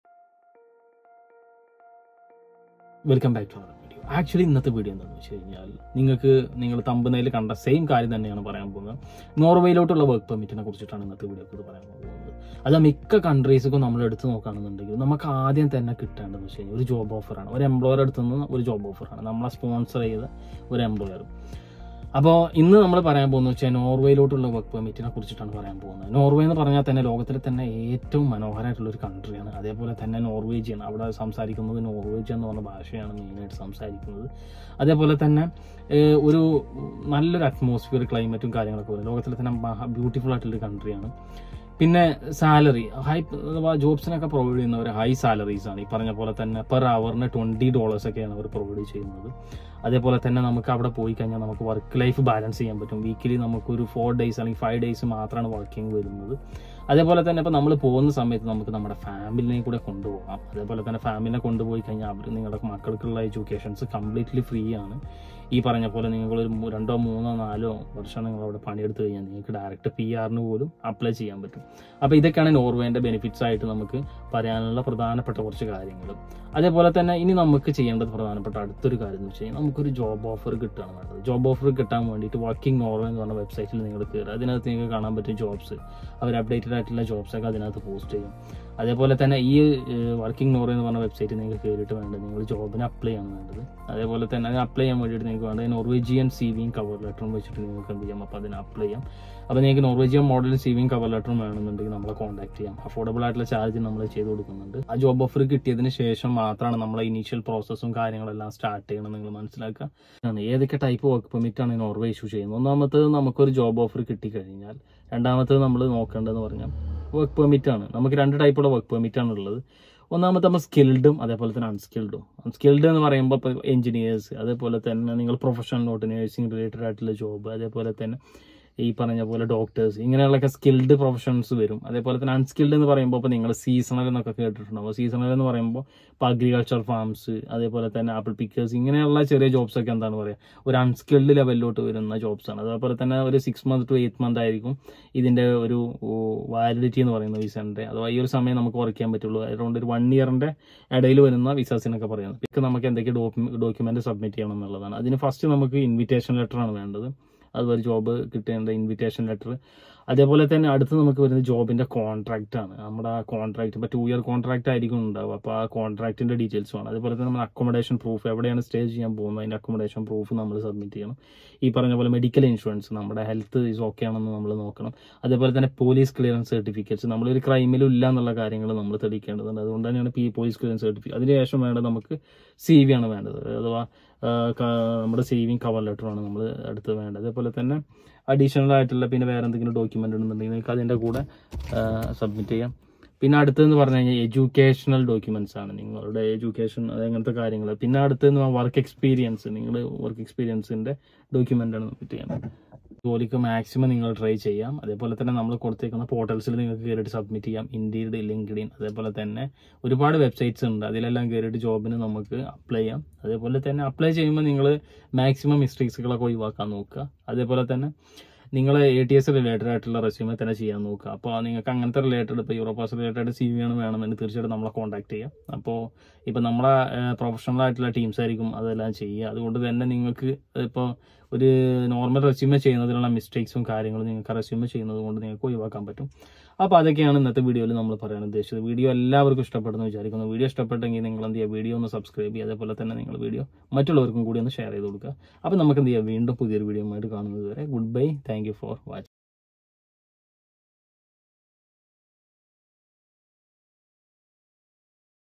Norway Work Permit Malayalam 2024 | Jobs in Norway | Full Process
Norway is a small nation in Northern Europe on the western side of the Scandinavian Peninsula that is both aesthetically stunning and economically advanced. Russia, Finland, and Sweden form its borders in the east and southeast. This region has enormous concentrations of natural resource reserves, including fish, gas, and oil, all of which raise living standards and promote national prosperity.
With approximately 5.5 million residents, Norway has a low unemployment rate of 3–4%. This is a very safe nation with excellent healthcare, excellent infrastructure, and free higher education. It is the perfect location for living, working, and conducting business.

Job Websites in Norway

Finn.no
Nav
Snaphunt
Jobbnorge
Indeed
KarriereStart
Jobzone
Adecco
Monster
Stepstone"

Job Assistance contact Whts up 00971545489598
CV Cover Letter Norwegian Format